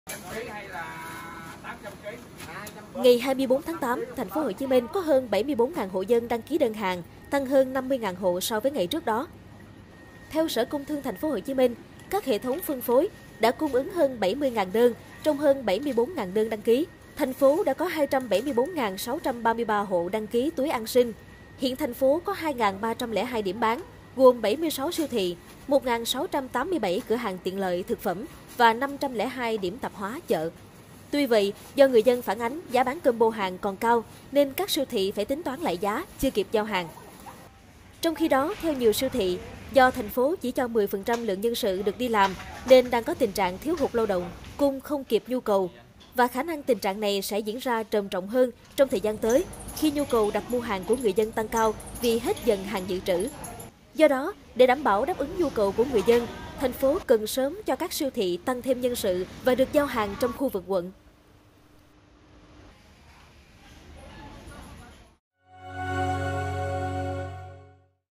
Hơn 74.000 hộ dân ở TP.HCM đăng ký đơn hàng, siêu thị sẽ tính lại giá combo
Ngày 24-8, TP.HCM có hơn 74 ngàn hộ dân đăng ký đơn hàng, tăng hơn 50 ngàn hộ so với ngày trước đó. Nhiều siêu thị cho biết sẽ tính lại các combo, do người dân phản ánh giá bán cao.